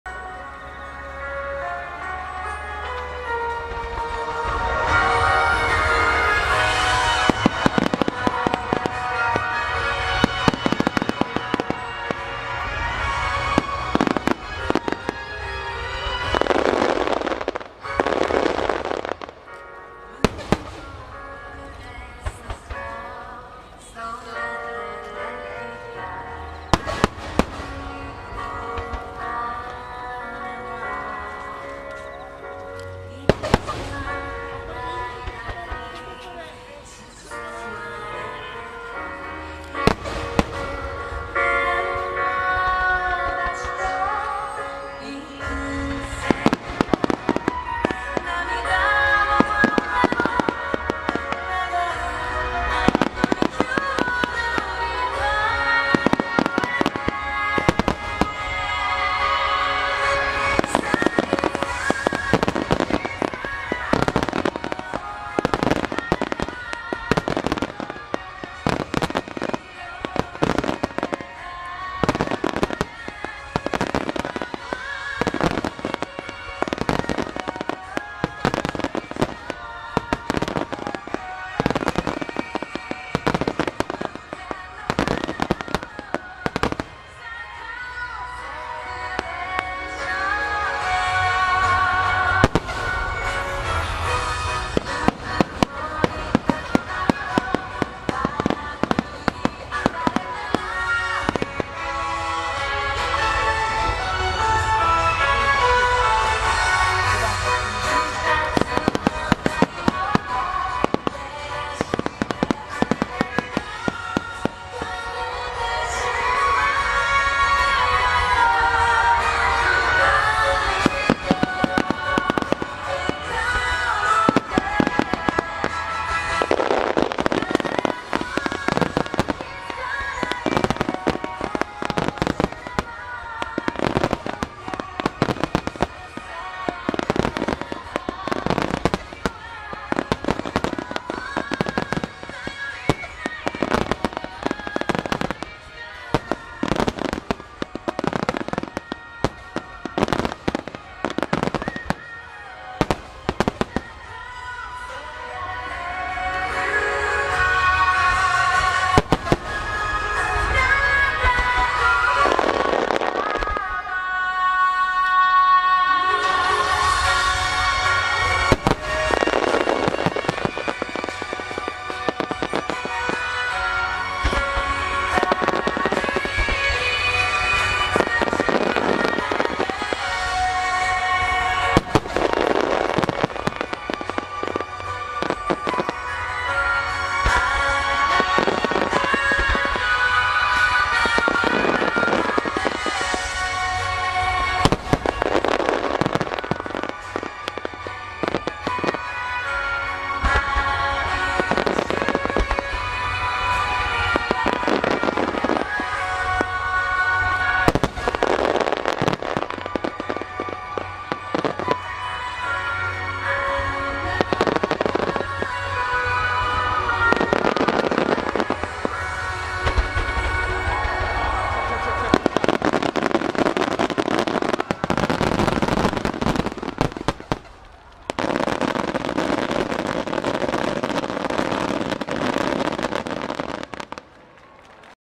[4K]赤川花火大会2019 エンディング後半 伊那火工堀内煙火店 🔴Akagawa Fireworks's Final shots by INA KAKO pyrotechnician
Name of Fireworks=Ephemeral bloom　一日の命の花
Time=令和元年８月17日 17th Aug 2019
Place=山形県鶴岡市赤川河畔Yamagata Pre Tsuruoka Shi Akagawa riverside.